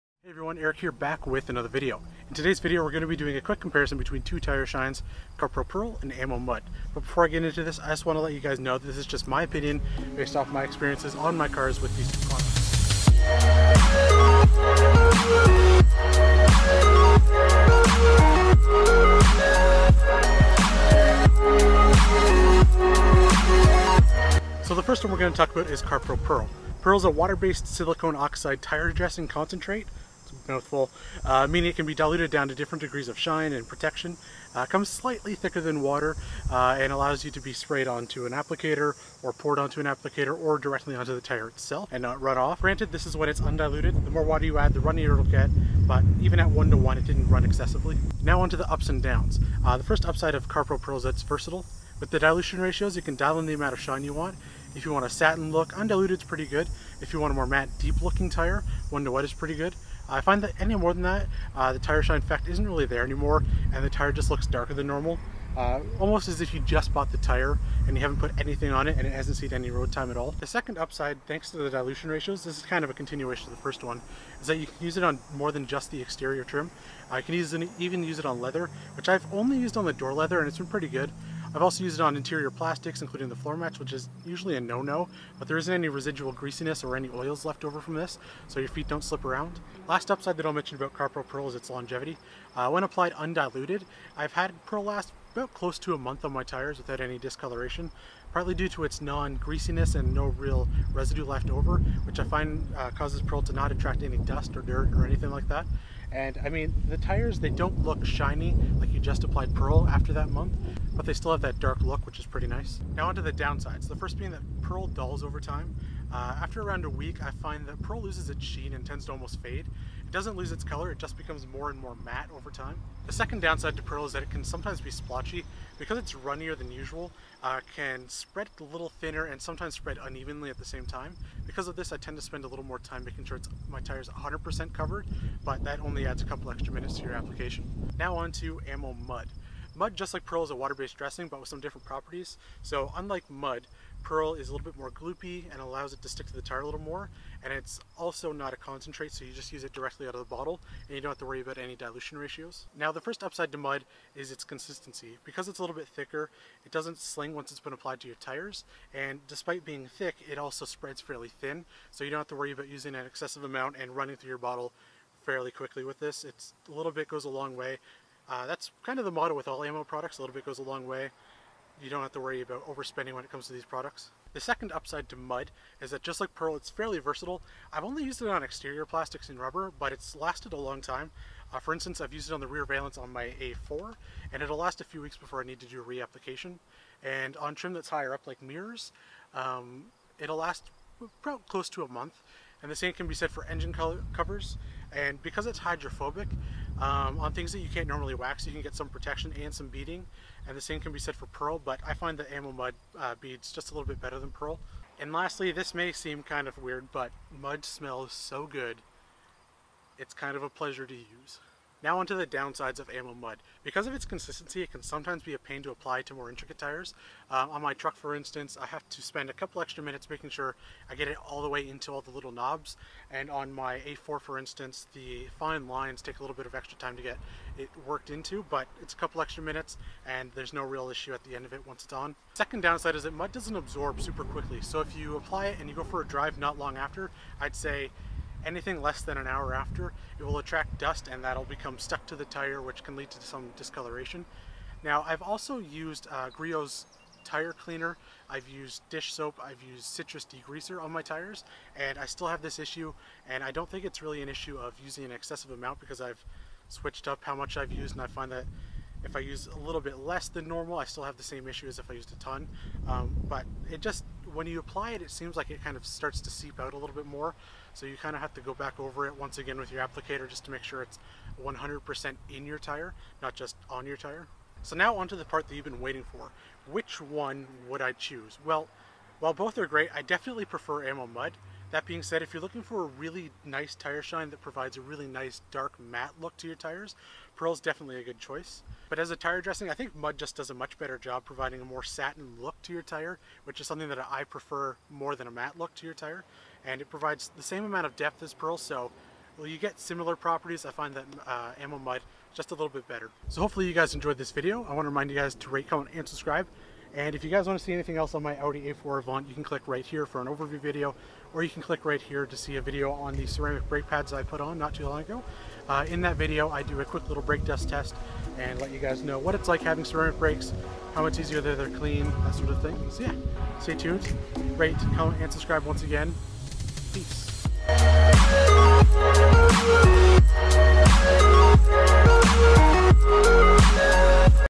Dual Review: AMMO Mud vs. CarPro Perl
Hey everyone thanks for checking out my review of AMMO Mud and CarPro Perl.